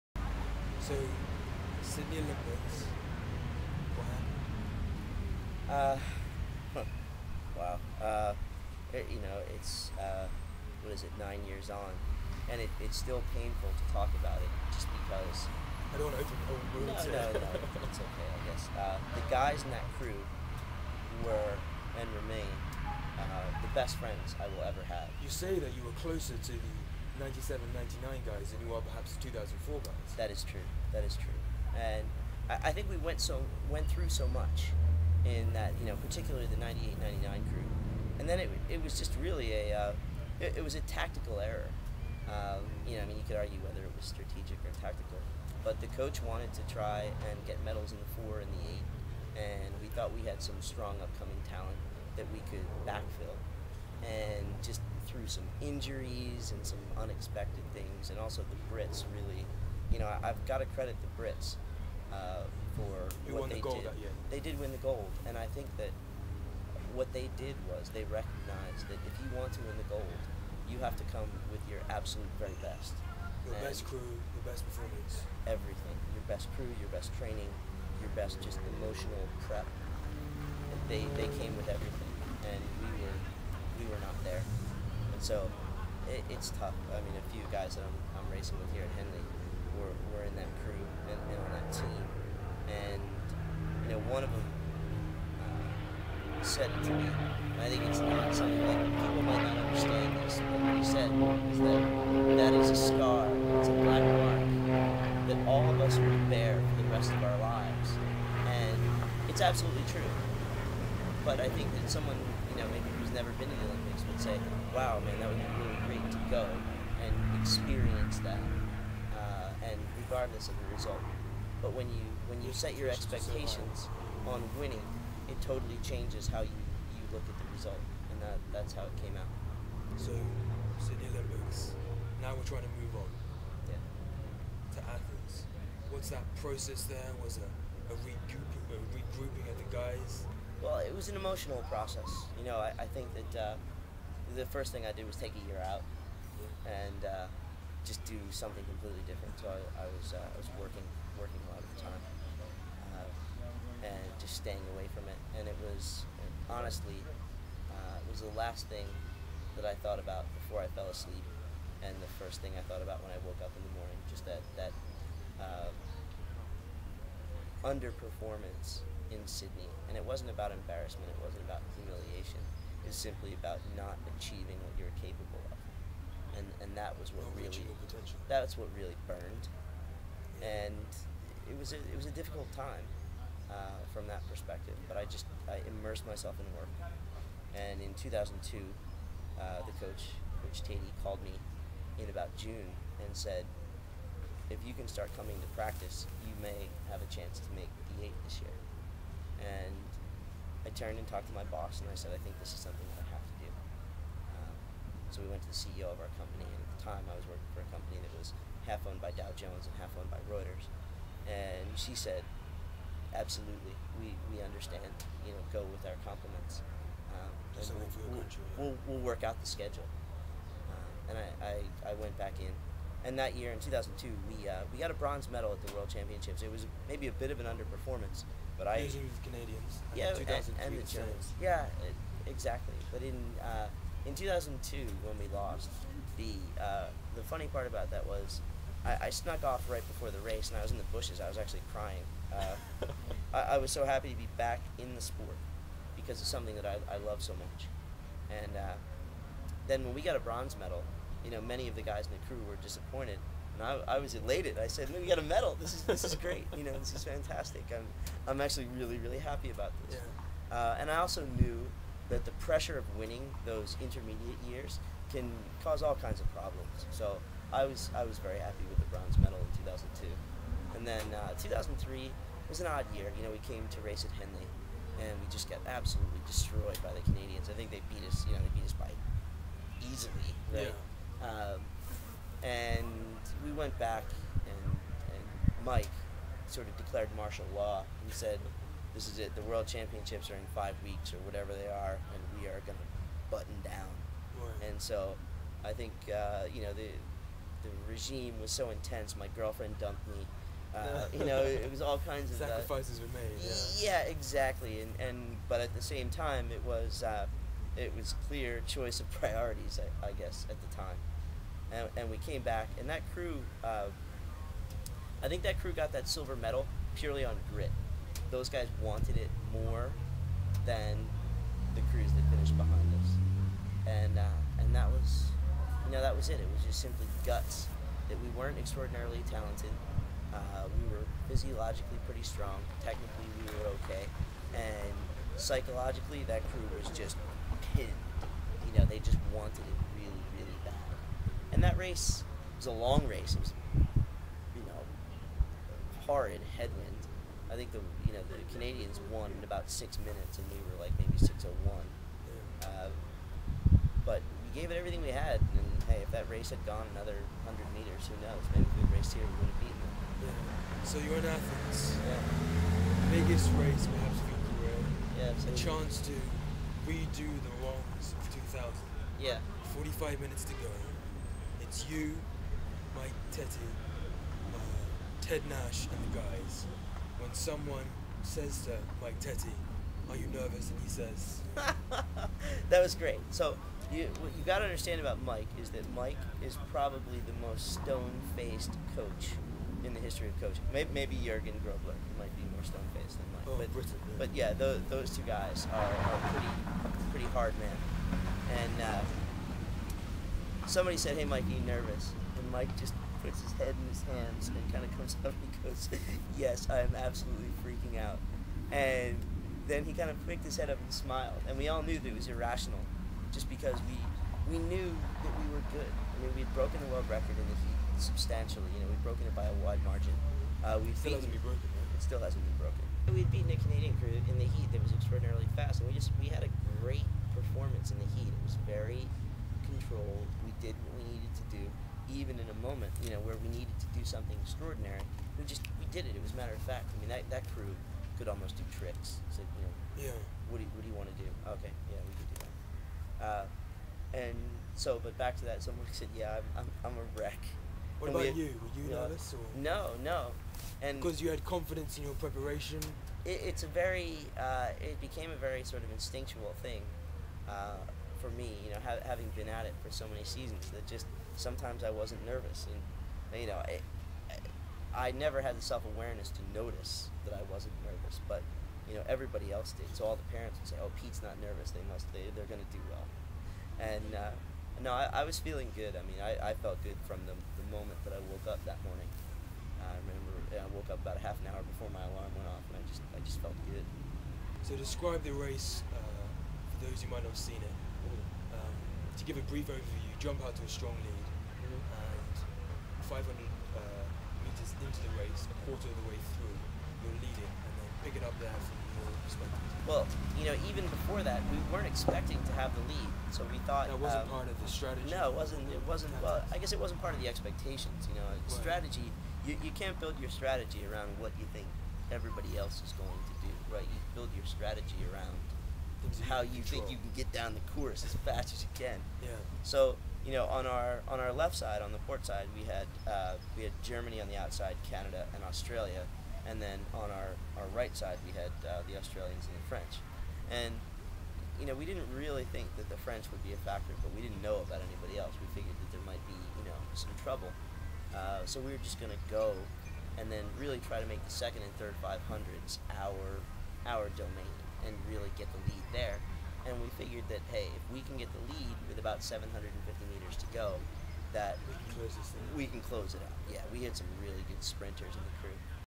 Conversations with DRAKE. Guest: Peter Cipollone. Part 3/5
A conversation with Peter Cipollone, Olympic gold medallist coxswain of the United States.